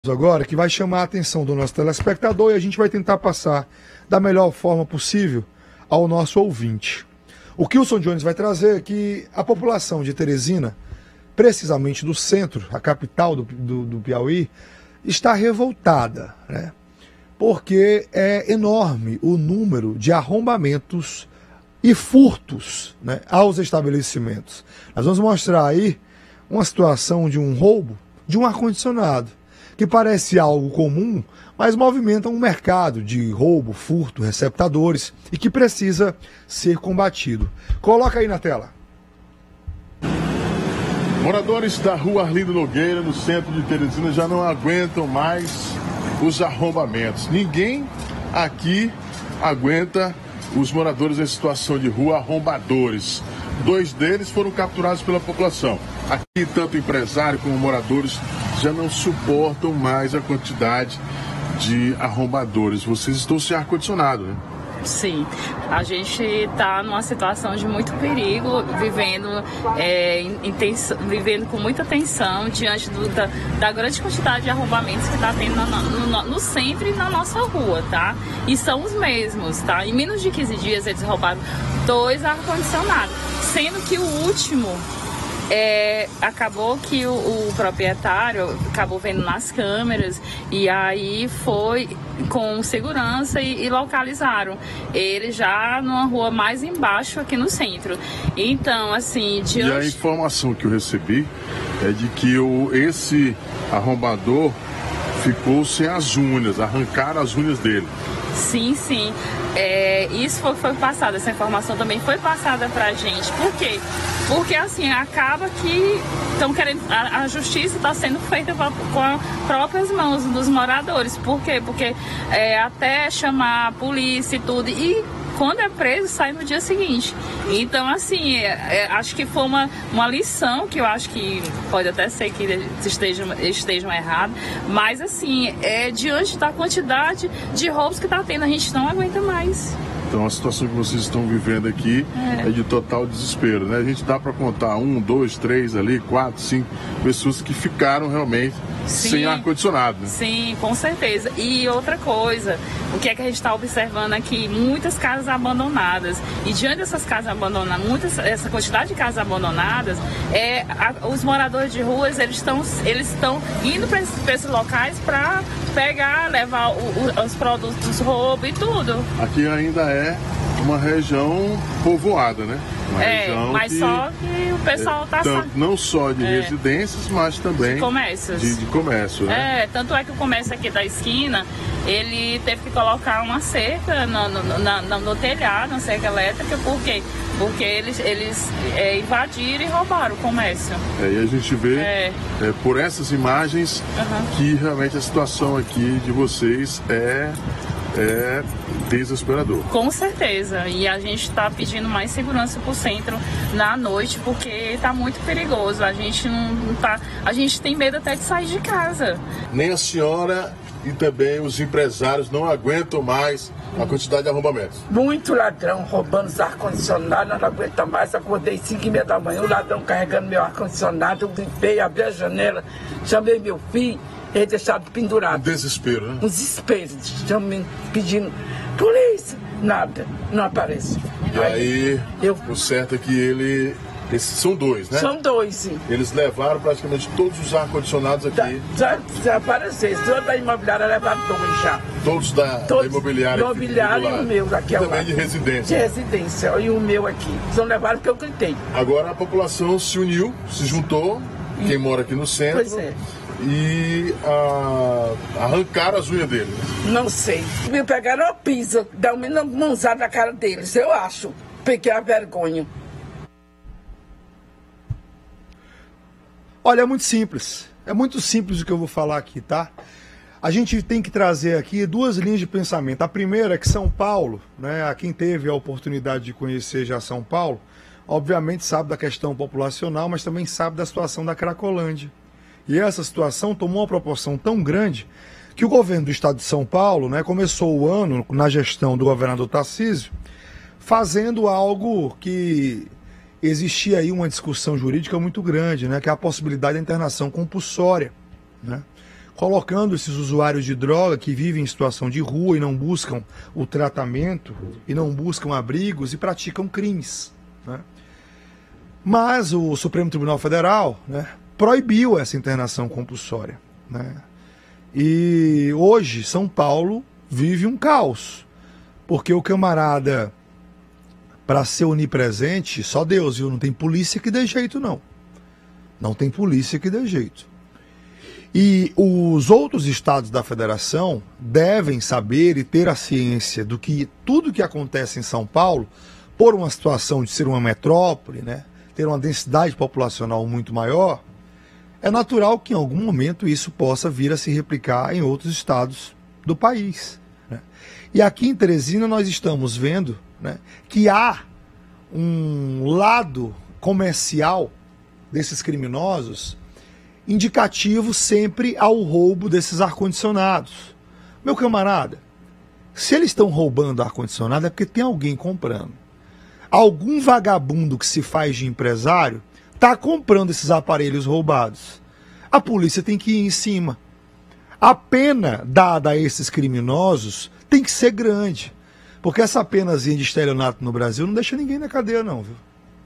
Arrombadores são capturados e tem unhas arrancadas por moradores no Centro de Teresina
TV Jornal é uma emissora de televisão brasileira sediada em Teresina, capital do estado do Piauí.